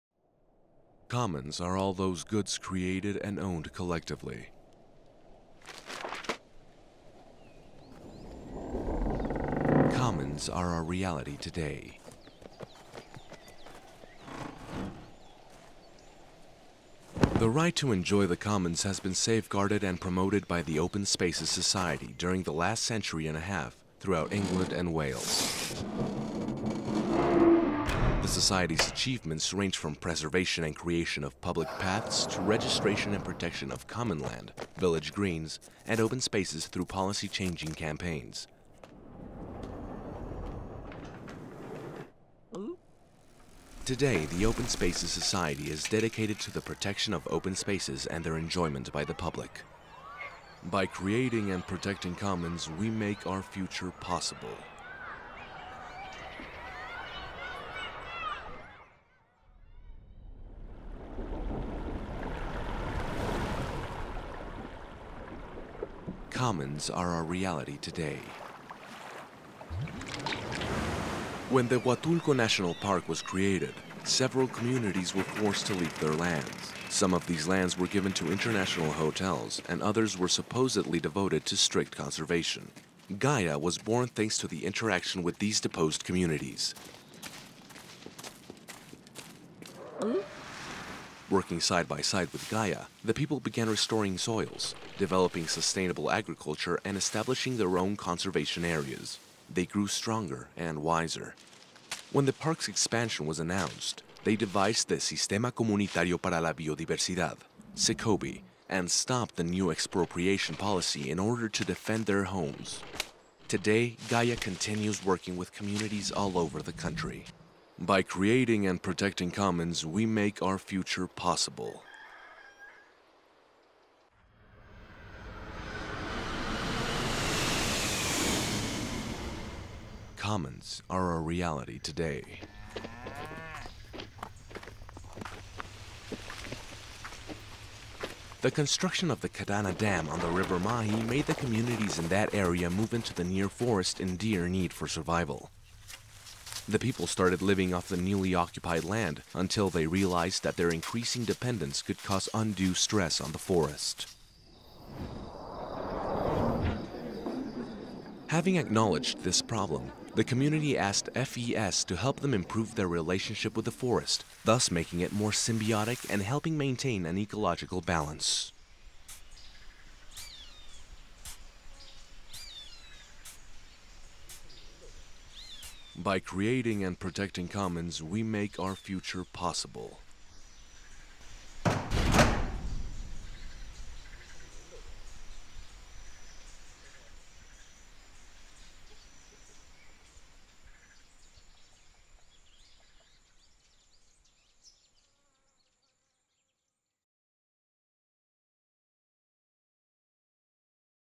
Commons in action
Commons are a reality today. By creating and protecting the Commons we make our future possible.

"Commons in action" is a joint program by the IASC and the Elinor Ostrom Award.

Join and support the commons!

The animation was done by the Viumasters. Get to know their work at viumasters.com